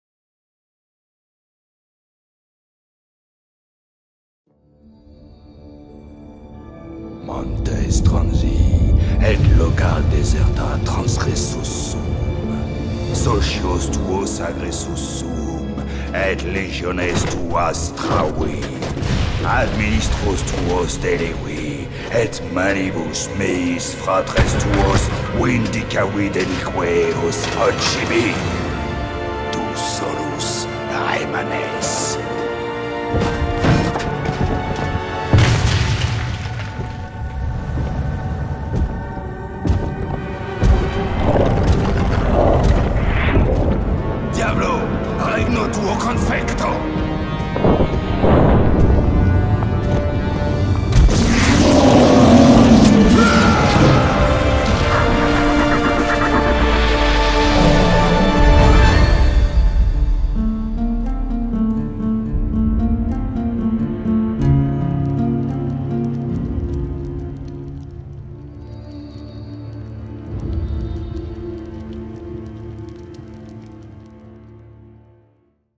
Diablo Fan Film Teaser
Project in stand-by because of lack of funding

This fan film (teaser), which was inspired by the Diablo novels, was lead by a team sharing the same passion for Diablo. Hoping you enjoy it.

Ce fan film (teaser), inspiré d'un des romans de l'univers Diablo, est un projet en cours mené par une équipe de passionnés et fans de Diablo. Il n'est soutenu par aucune boite de production et totalement autofinancé. En espérant que cela vous plaise. Restez informé en vous abonnant à ma chaîne YouTube.

Traduction pour mes amis français:

J'ai traversé les montagnes. Franchi les déserts.
Affronté tes alliés. Terrassé tes légions.
Détruis tes séides. Ma main vengeresse a occis tes frères.
Ne reste que toi.
Diablo! Ton règne s'achève!

Actor - Yann Salim Abidi Dominique Duche

Director of photography - Vitalijus Kiselius

CG surpervisor - Amaury Brunet "Looking for job"

Matte Painter / previs artist / moodboard creator : Rodolphe Parfait

Storyboarder - Vincent Français

Making Of - Frederic Tardieux

Forger - Dominique Duche

Sound Designer / mixing - Quentin Roudil

Key Grip - Laurent Wallerang

Grip / Electrician - Anthony Schaack, Pierre Bellion, Ansel Arbogast, Christophe Bertaux

Makeup FX - Thibault Franquin

Voice Recorder - Yann Jenny

Narrator / Voice - Olivier Piechaczyk

Additional Visuel Effects - Julien Bartholini, Anthony Schaack, Christophe Bertaux

English Translator - Stella Cuscé

Latin Translator - Abbe Mathieu Siebel

Special Thanks to: Treve Dance Center, Mantas Satkus, Michel Dimmer, Stephane Caboche, CNA Luxembourg, Ken Fitzke, Pierre Alt